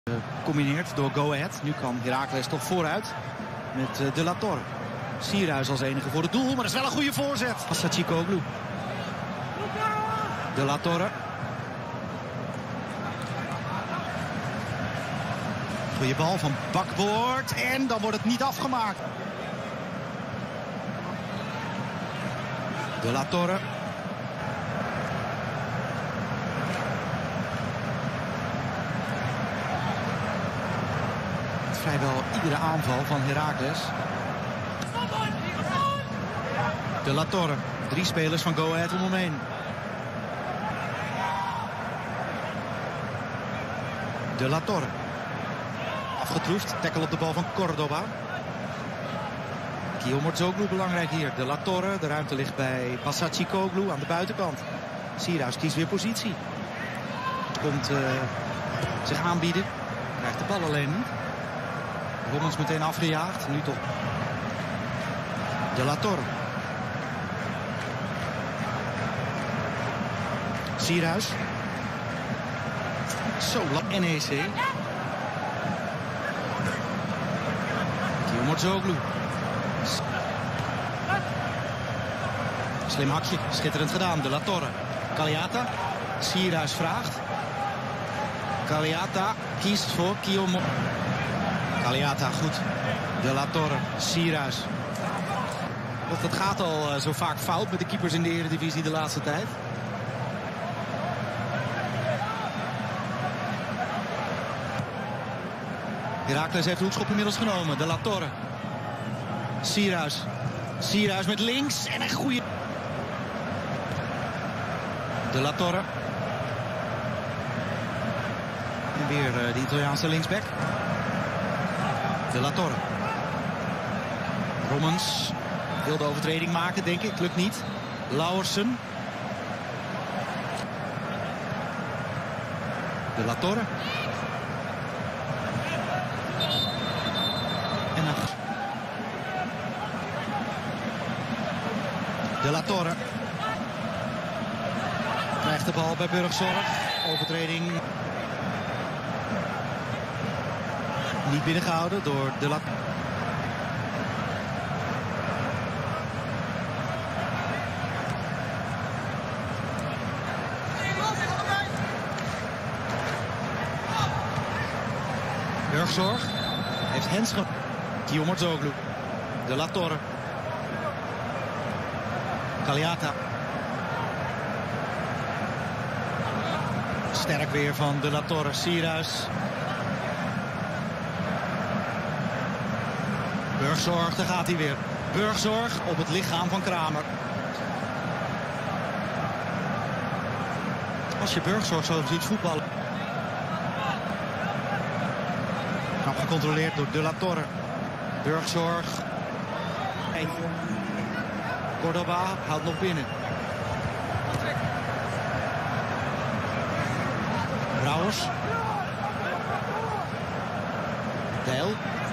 Luca de la Torre vs Go Ahead Eagles
Luca de la Torre's performance from this weekend. Another tidy performance before heading to USMNT duty.